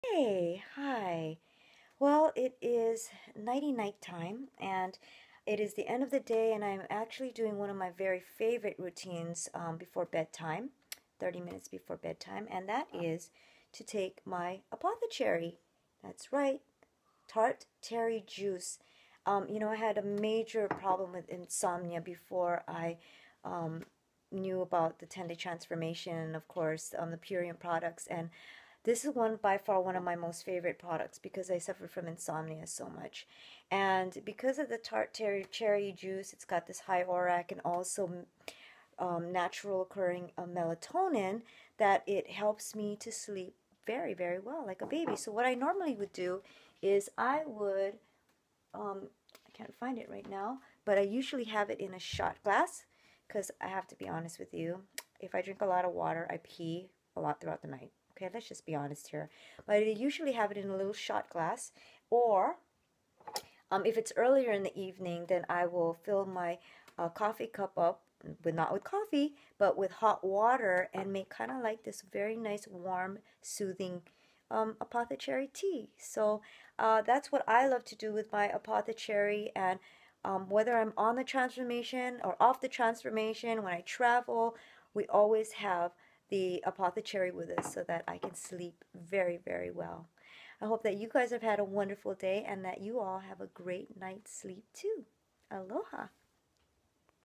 Purium's Apothocherry
Purium's Apothocherry is great for a good night's rest!
Insomniacs would love this!